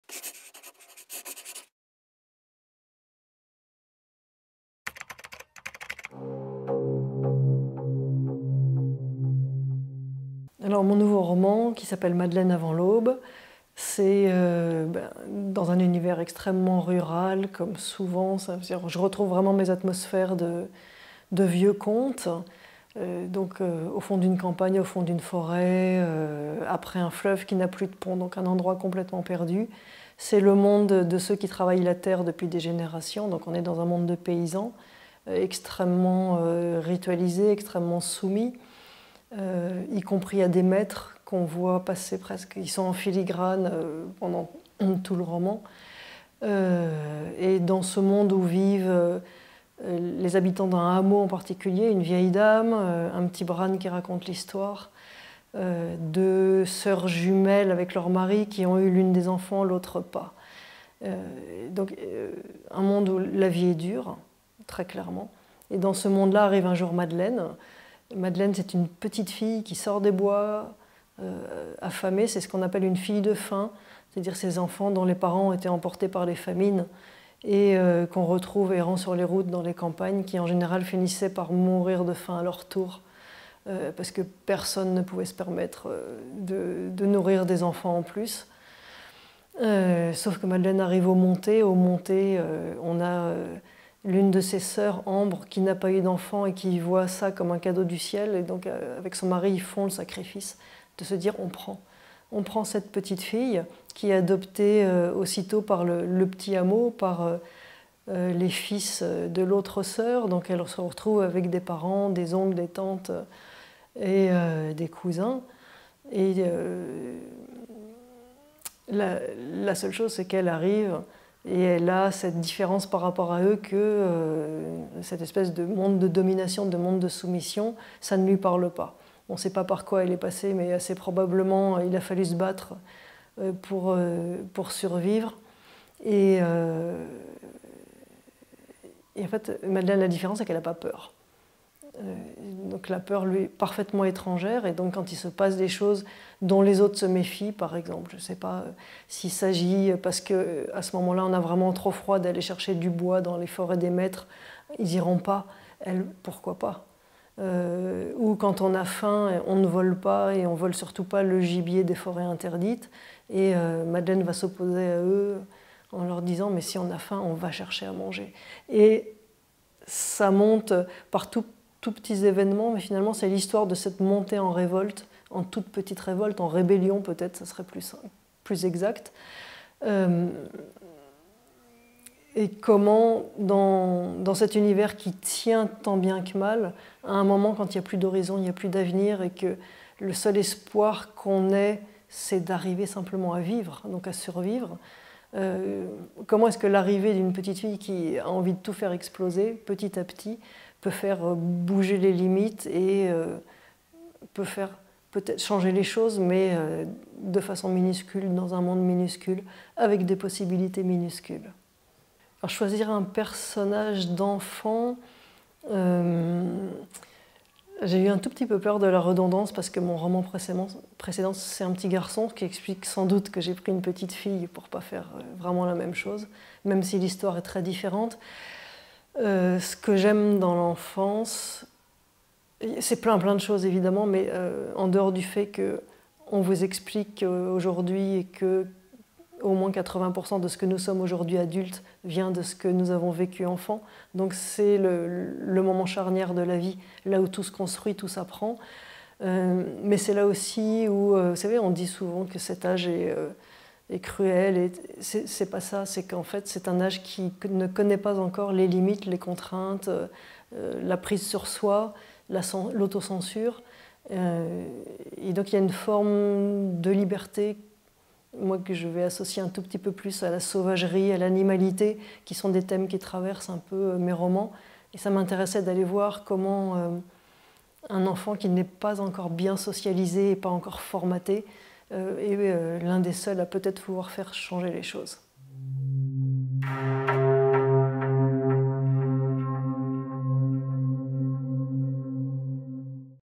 Sandrine Collette - Madelaine avant l'aube
Sandrine Collette vous présente son ouvrage "Madelaine avant l'aube" aux éditions JC Lattès. Rentrée littéraire automne 2024. Prix Goncourt des Lycéens 2024.